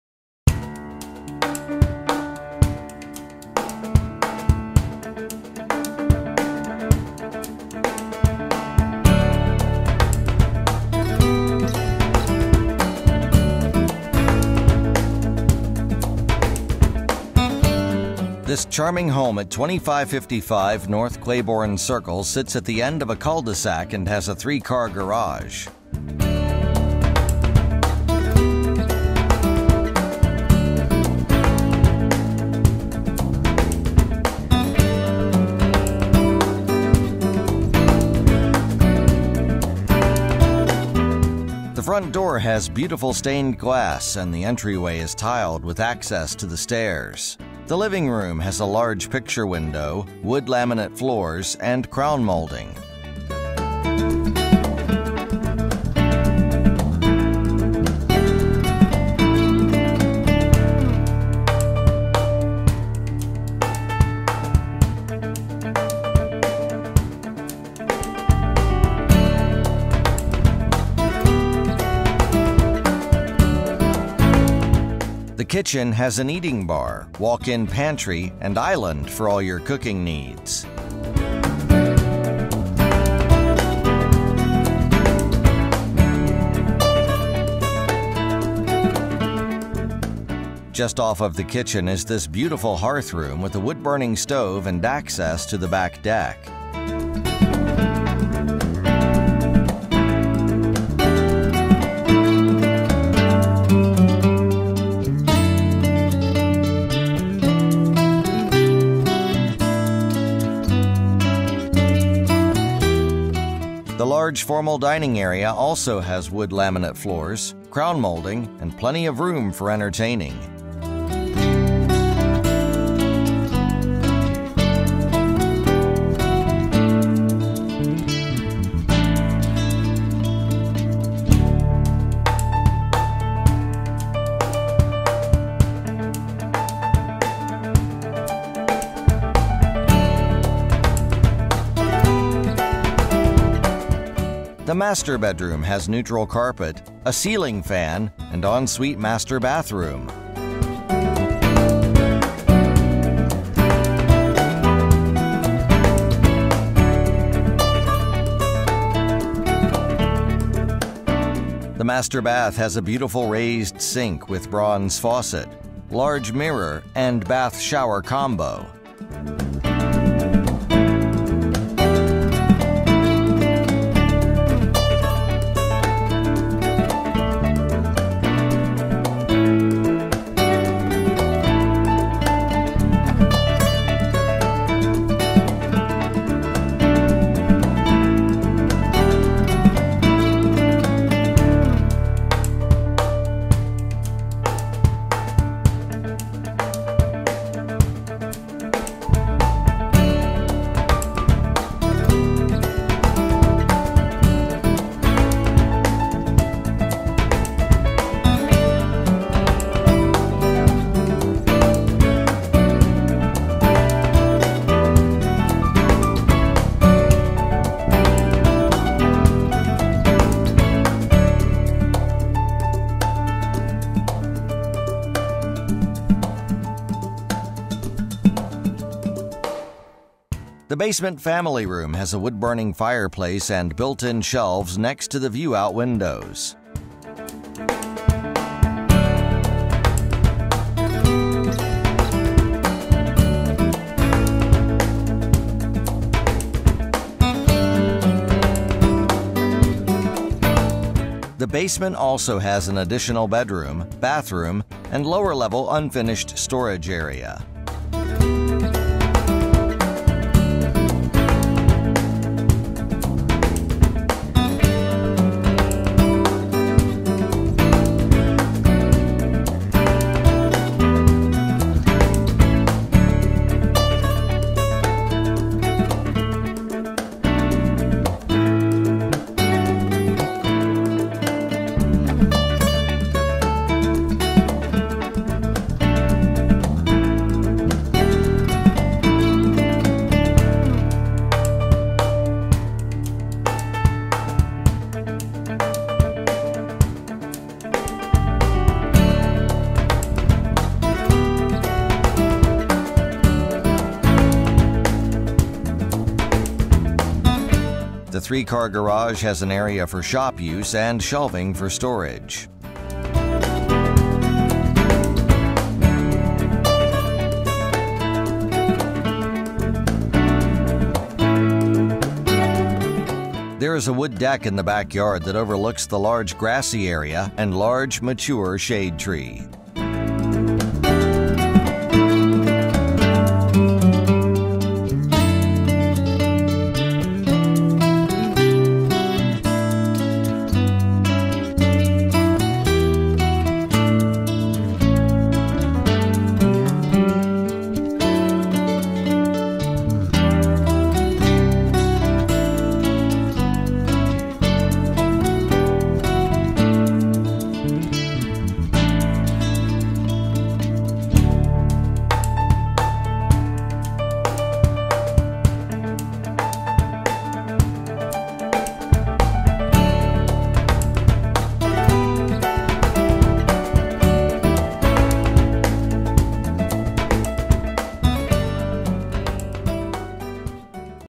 2555 N. Claiborn Circle Presented by Coldwell Banker Plaza Real Estate and Heather Stewart
Wichita are real estate for sale by Heather Stewart, Coldwell Banker, Wichita KS
This all electric home offers many extras. Updated kitchen with stainless steel appliances & walk in pantry with butcher block island & eating bar overlooking the hearth room leading to the partially covered wood deck. Formal dining & large living room. Updated bathrooms with master half bath adjoining the hall full bath for convenient shower/bath access. View out lower level with rec. room featuring a wood burning fireplace. Lower level is unfinished & offers lots of space for storage or the ability to finish for even more space, such as office, bonus room, game room, etc. Fantastic oversized 3 car tandem garage with large workshop is a delightful surprise. Large privacy fenced backyard with mature trees is fairly private with sunshade & swing. Nearby area playground is another added bonus. HOA covers multiple playgrounds, a sand volleyball court, a soccer field, lighted paths, summer cookout & neighborhood disposal day. Located on a cul de sac lot this home offers a lot of value for the money.